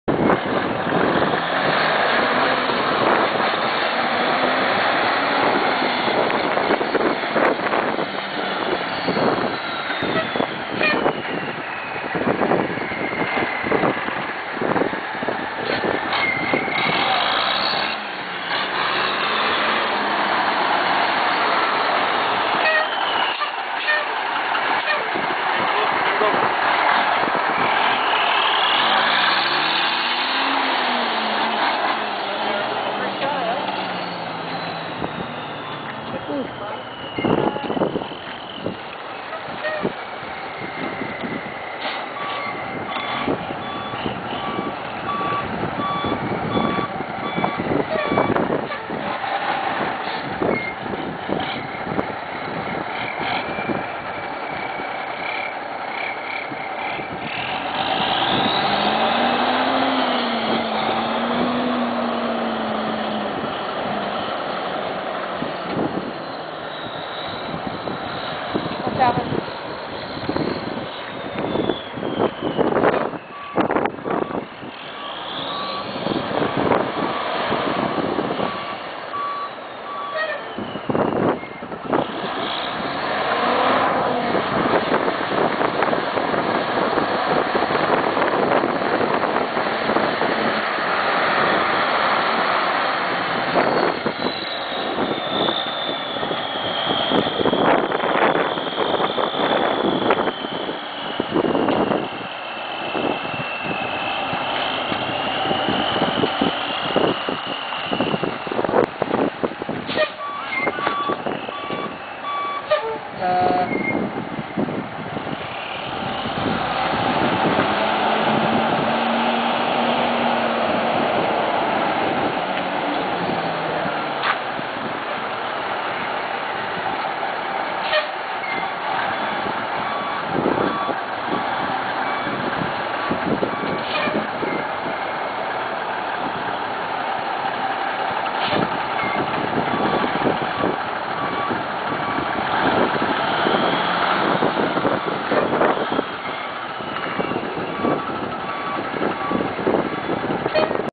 Dump Truck Maneuvering 2
GCA Construction & Truck Rodeo Day 1 at the Guam International Raceway in Yigo. February 27, 2010.